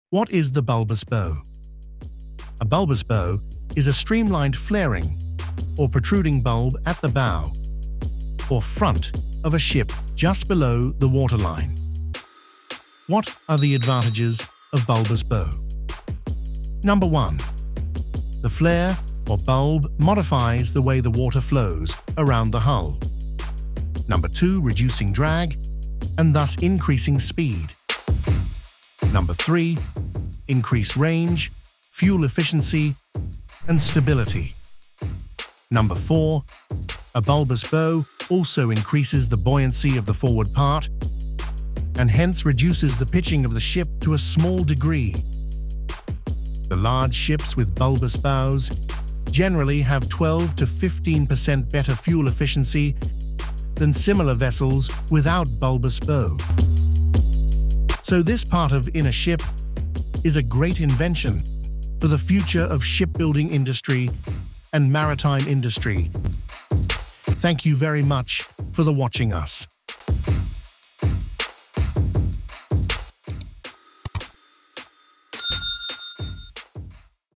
What's the bulb💡⚙️protruding from the front of the ship?🤔😲🤩
What is the bulbous bow?💡🚢

A bulbous bow is a streamlined flaring or protruding bulb at the bow (or front) of a ship just below the waterline. 🤩🤩

What are the advantages of bulbous bow🤔?

The flare or bulb modifies the way the water flows around the hull, reducing drag and thus increasing speed, range, fuel efficiency, and stability.😲🤩😍

 Large ships with bulbous bows generally have 12% to 15% better fuel efficiency than similar vessels without bulbous bow.
A bulbous bow also increases the buoyancy of the forward part and hence reduces the pitching of the ship to a small degree.